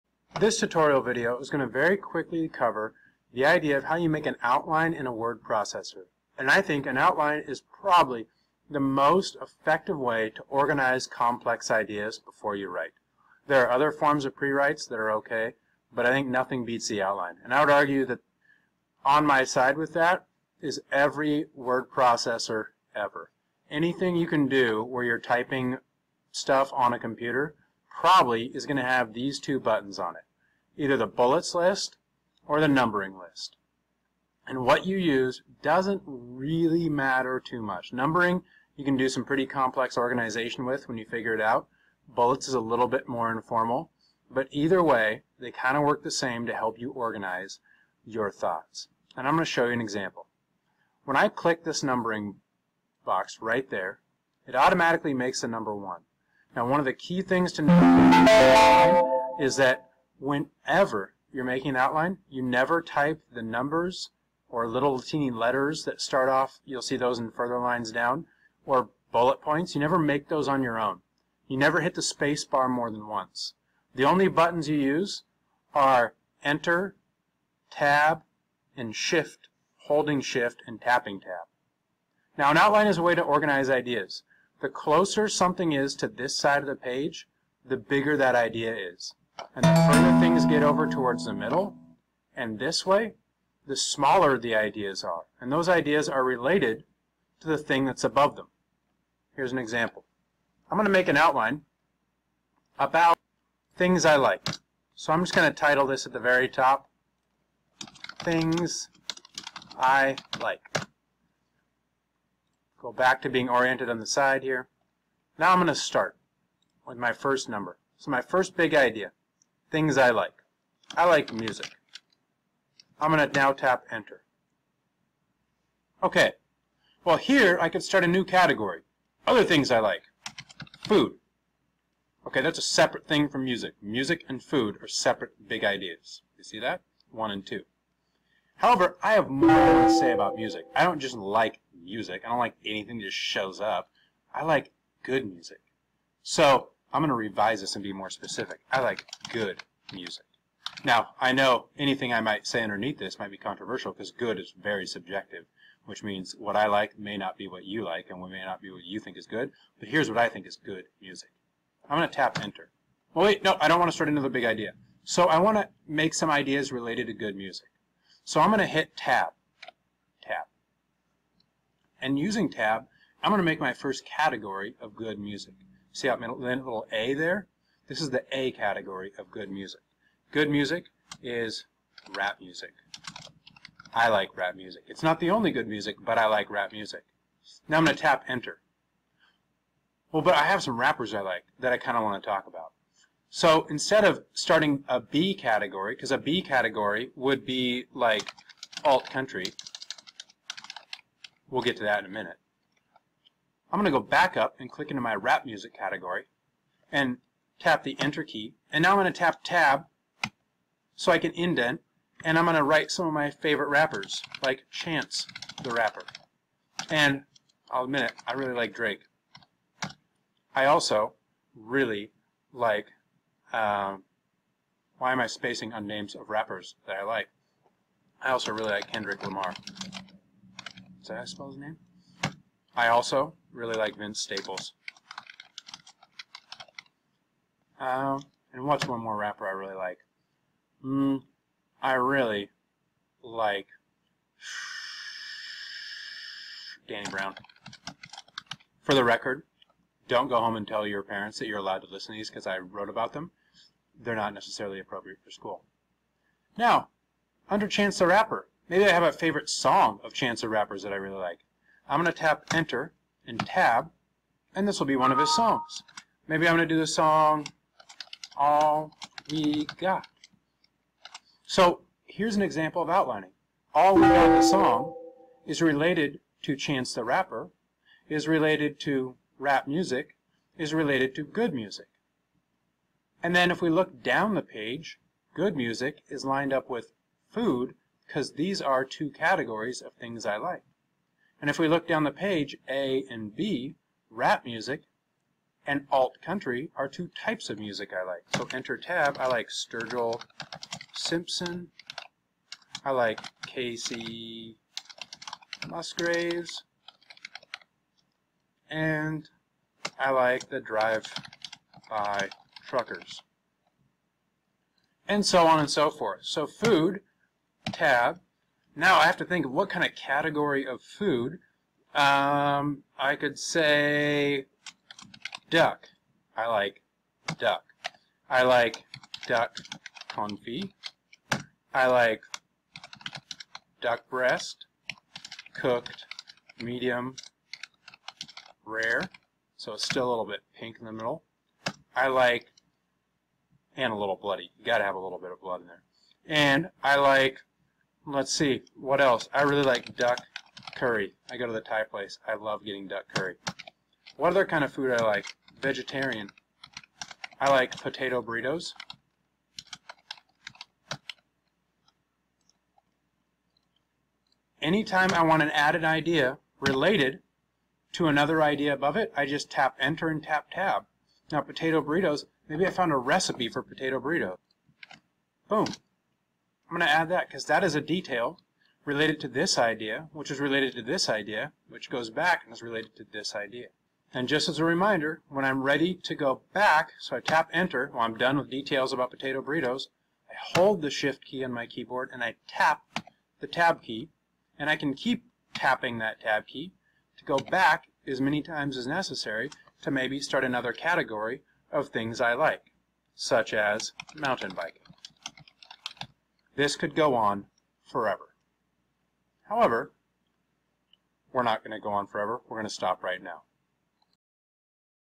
How to Outline on Microsoft Word Online and Most Other Word Processors
A tutorial video I made for my blended learning class showing how to organize ideas in outline format using a word processor.